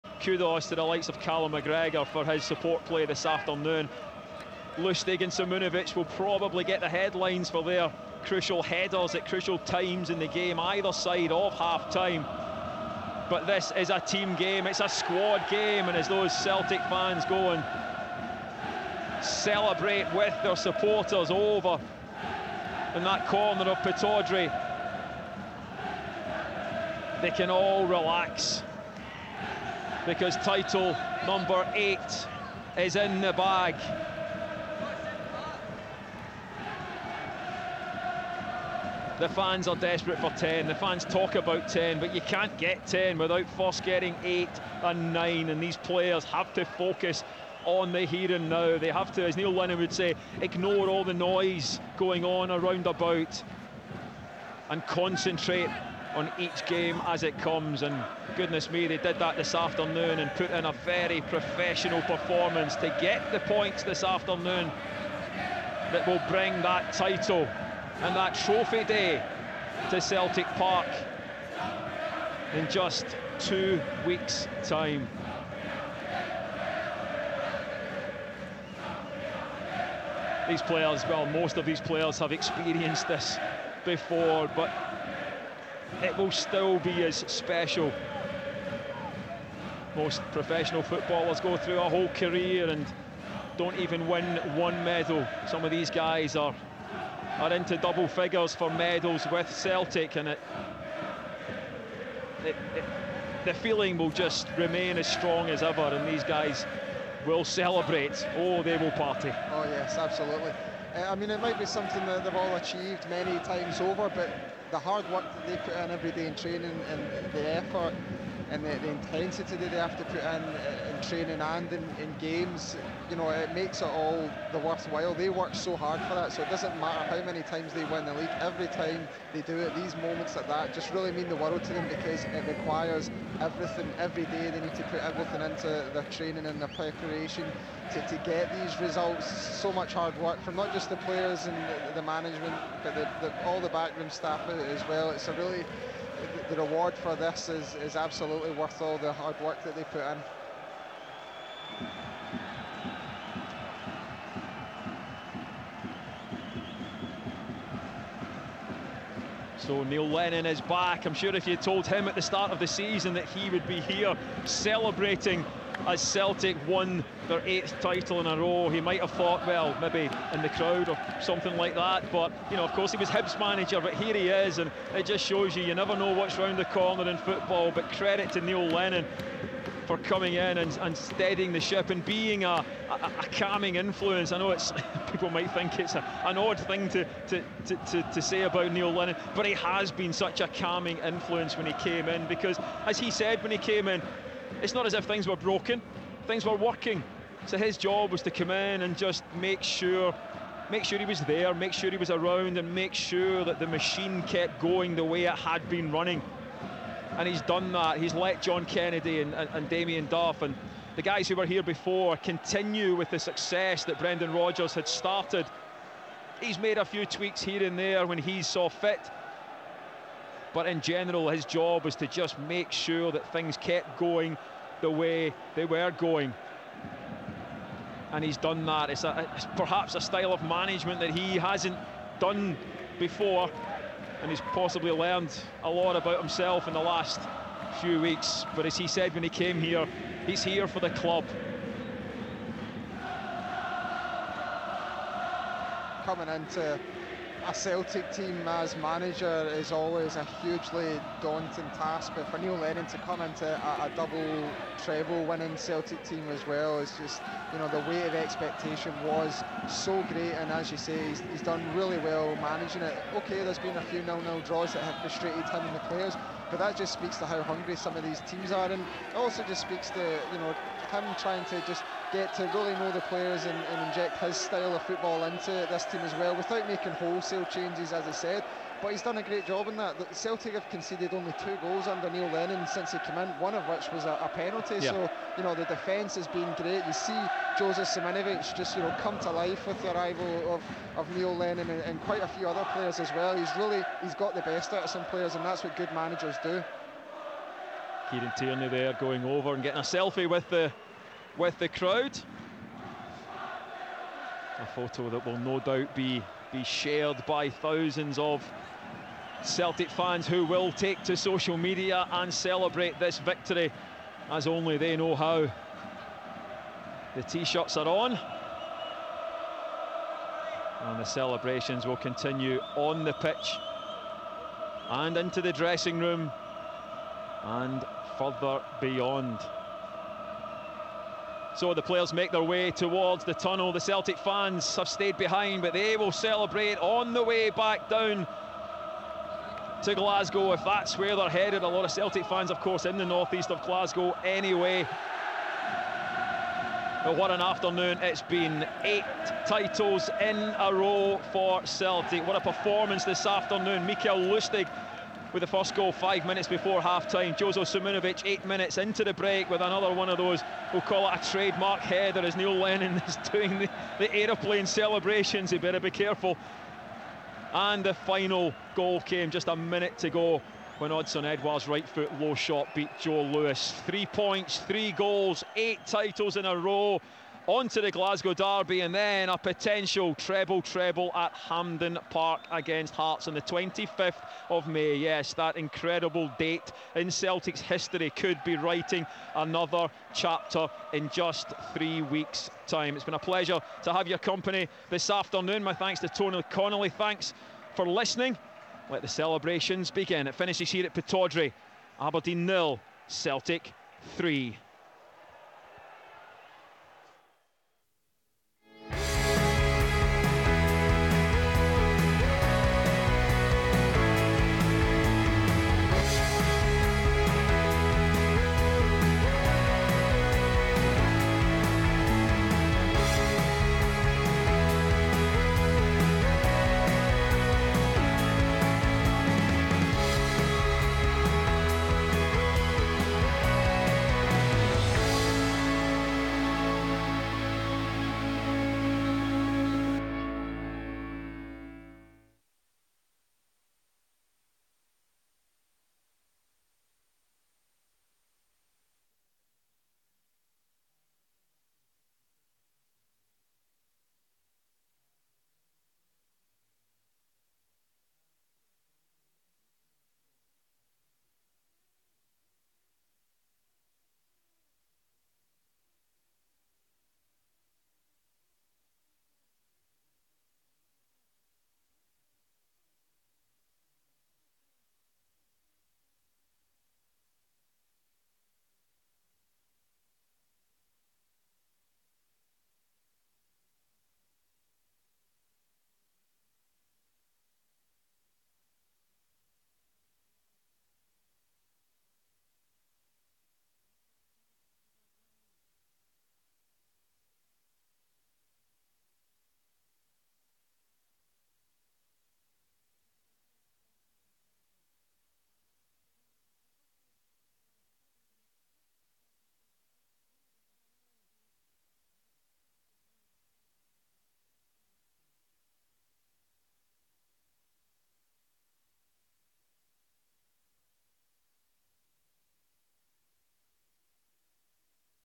Post match celebrations from Pittodrie.
Celtic TV live from Pittodrie Stadium.

Pictures courtesy of Sky Sports